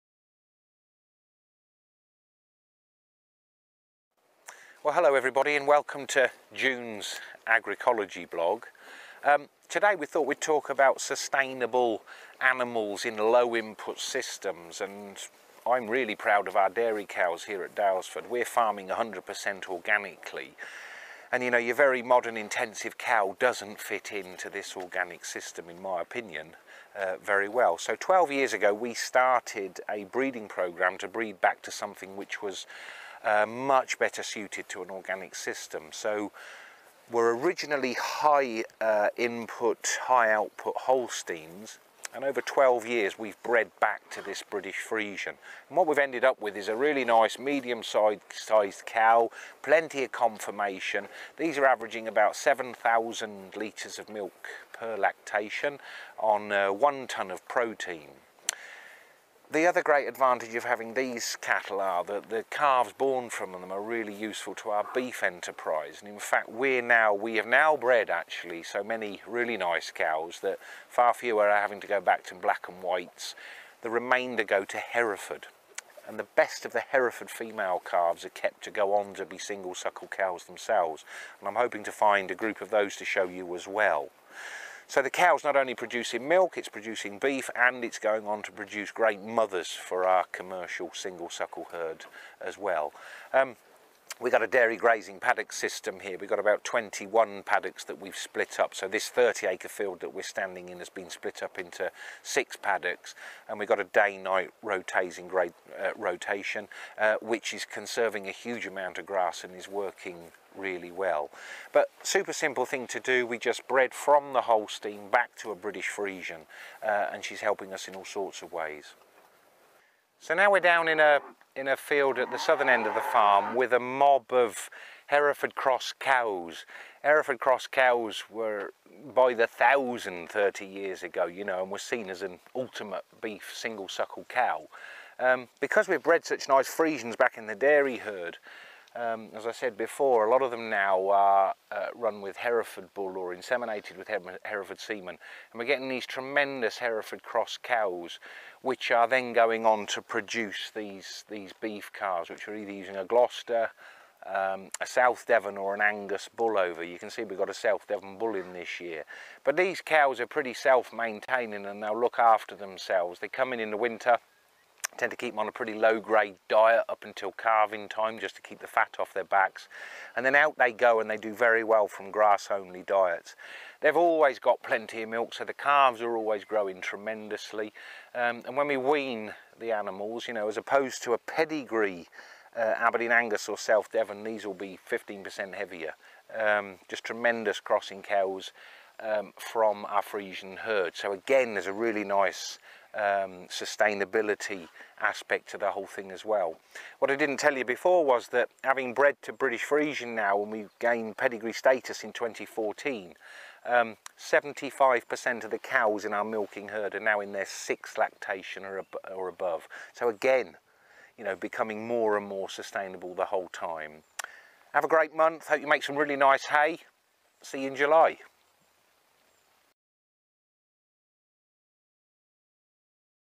Breeding a sustainable dairy cow
In June's Vlog, Richard explains how a sustainable dairy cow has been bred at Daylesford Farm.